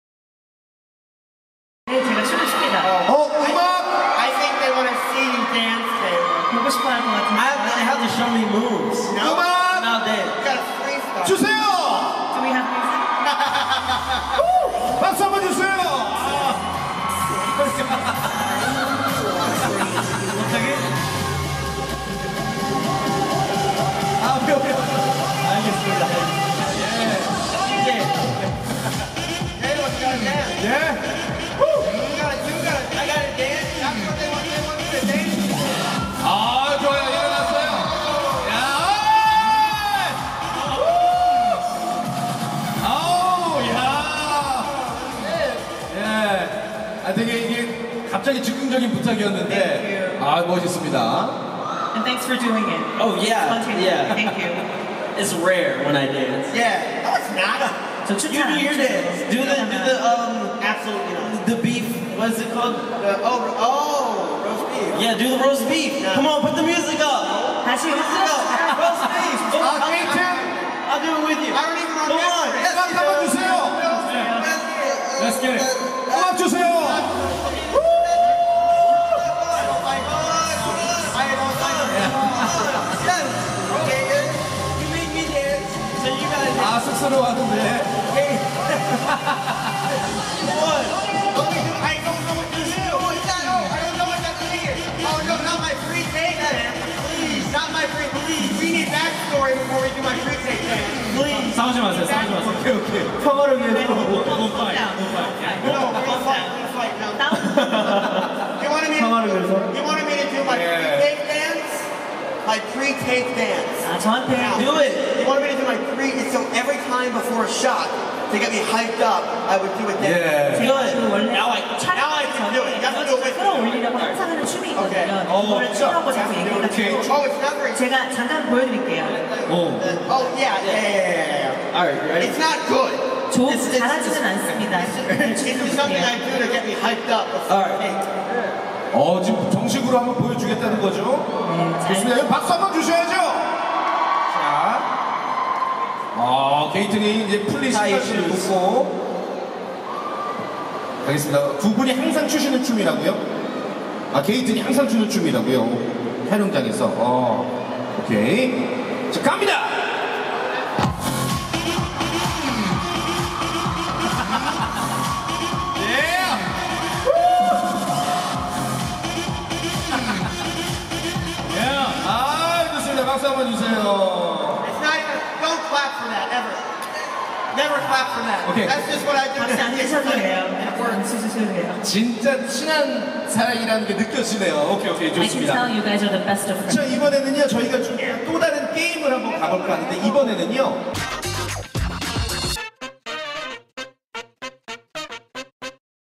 190620 루카스X더스틴, 흥 돋는 댄스타임 ('기묘한 이야기 3' 레드카펫)
20일 오후 서울시 영등포구 타임스퀘어에서 넷플릭스(Netflix) 오리지널 시리즈 '기묘한 이야기 3'의 레드카펫 행사가 열려 배우 게이튼 마타라조(더스틴 역)와 케일럽 맥러플린(루카스)와 엑소(EXO) 수호와 카이가 참석했다.

게이튼 마타라조(더스틴 역)와 케일럽 맥러플린(루카스) 이 댄스 실력을 선보이고 있다.

한편 넷플릭스(Netflix) 오리지널 시리즈 '기묘한 이야기 3'는 1985년 여름 호킨스 마을에서 벌어지는 더욱 기묘해지고 거대한 사건을 다룬 미스터리 스릴러 작품으로 오는7월 4일 공개된다.